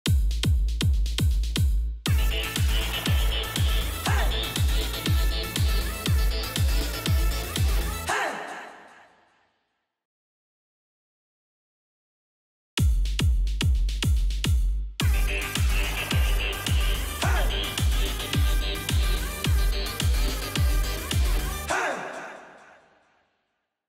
これがこう AI, a cat ear fantasy girl. 猫耳ファンタジー女子アイちゃん。60fps
BGM TONIKAKU-like Celtic music.